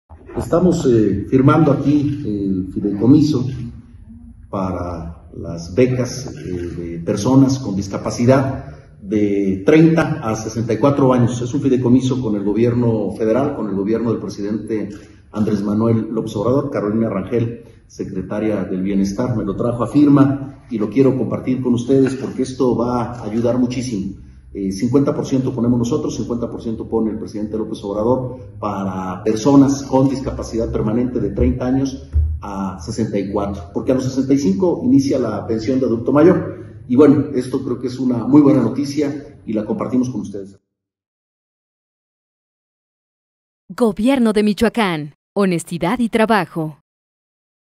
Trabajamos por el bienestar de los grupos vulnerables para que tengan una vida digna
En compañía de Carolina Rangel, titular de la Secretaría del Bienestar, firmamos un fideicomiso con el Gobierno Federal para el financiamiento de las becas de personas con discapacidad de 30 a 64 años, que contribuye a disminuir la brecha de desigualdad.